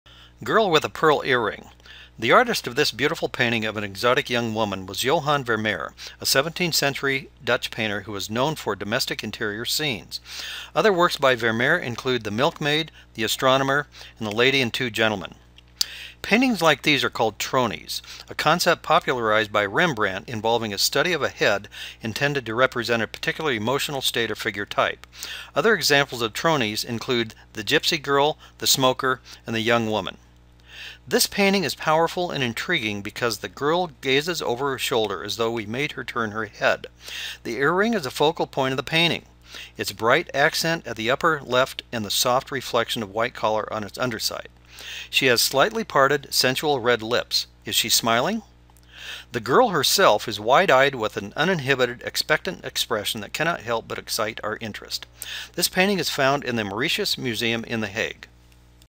Girl with the Pearl Earring
Overview of classic painting produced by James Carey, UC Davis